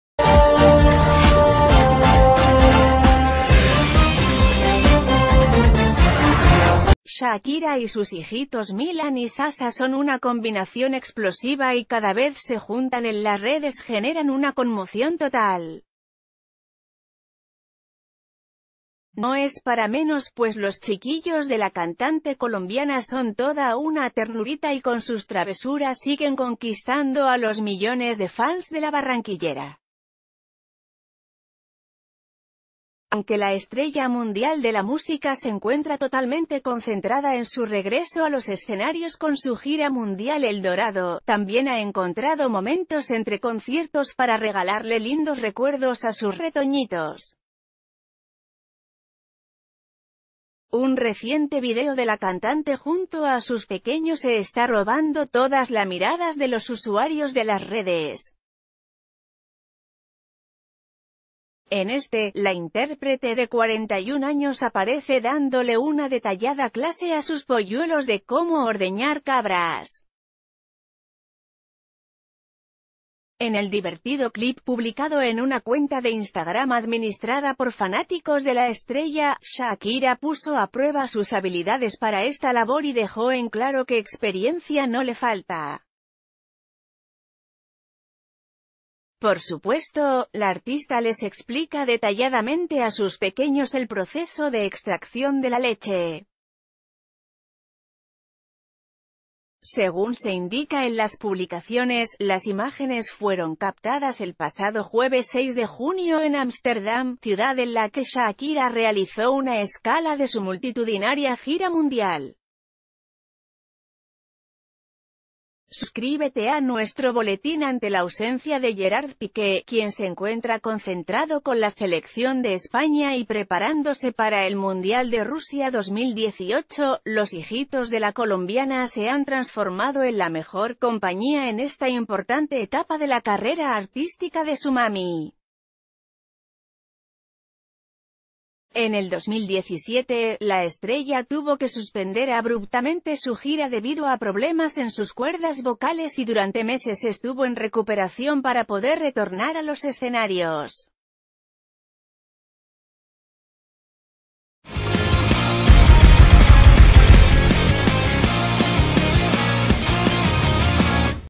Shakira le enseña a sus hijos a ordeñar cabras
Shakira y sus hijitos Milan y Sasha son una combinación explosiva y cada vez se juntan en las redes generan una conmoción total. No es para menos, pues los chiquillos de la cantante colombiana son toda una ternurita y con sus travesuras siguen conquistando a los millones de fans de la barranquillera...